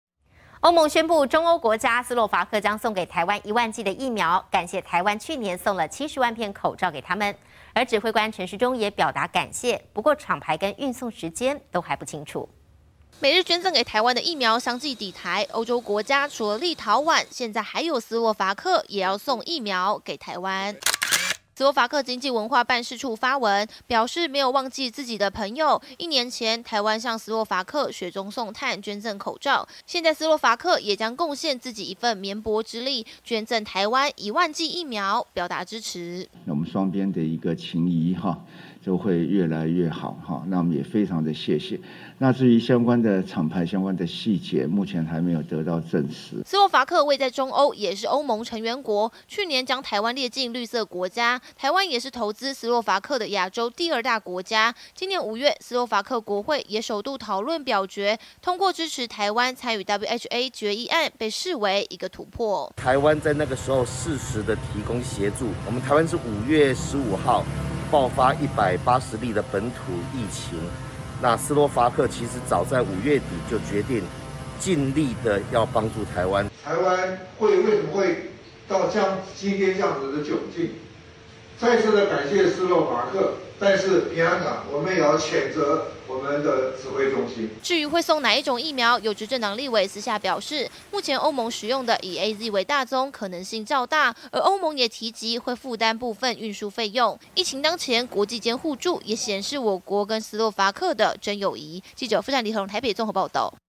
贈台疫苗 斯洛伐克辦事處:沒忘自己的朋友
歐盟宣布，中歐國家斯洛伐克將送給台灣一萬劑疫苗，感謝台灣去年送了70萬片口罩給斯洛伐克，而指揮官陳時中也表達感謝，但廠牌跟運送時程，坦言還不清楚。